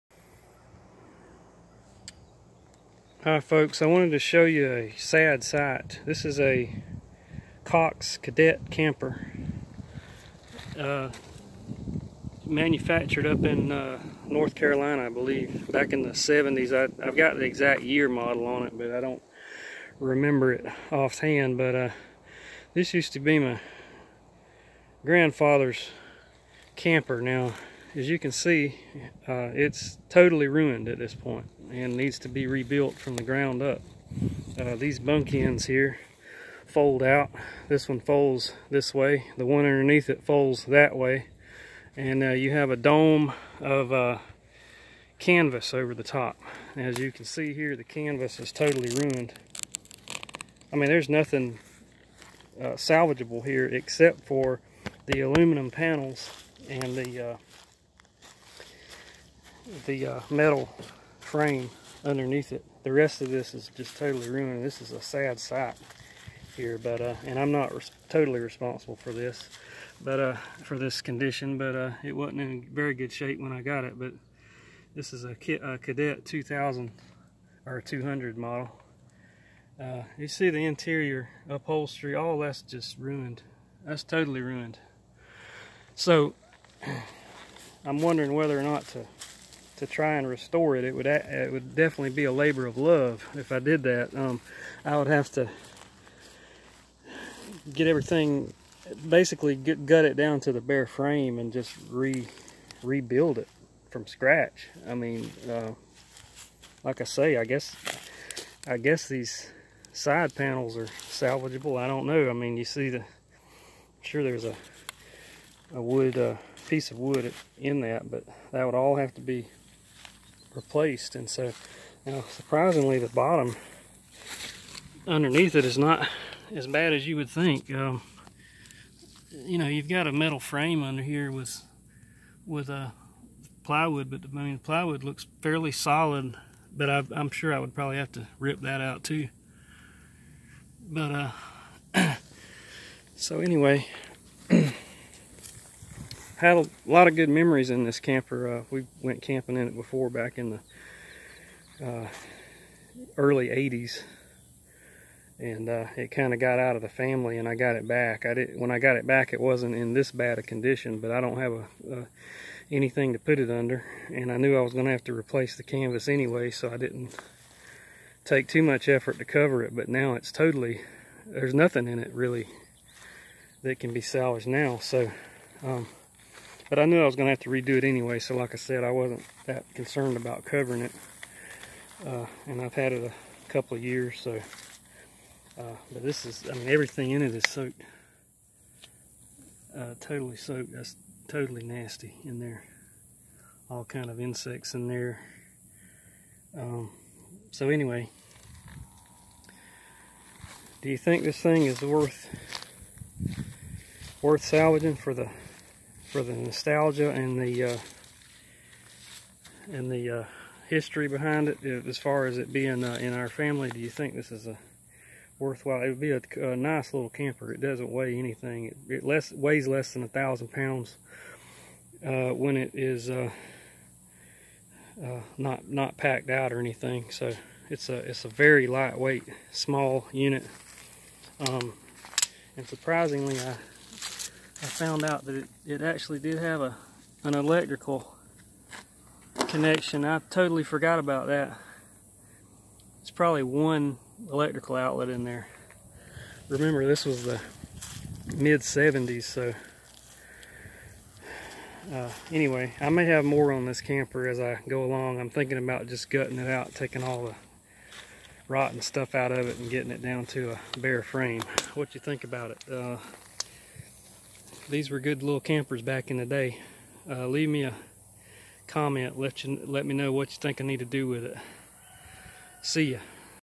Neglected Cox Cadet camper Pt 1
Some history:  my grandfather bought this series 200 Cox Cadet camper used in the late 70's or early 80's. He owned it into the 90's when he sold it to a friend.  The friend gave it to me,  considering it's condition,  about 3 years ago.  I haven't done anything with it.  This is a look at the water damage and the current condition. I'd like to restore it but it would definitely be a big job.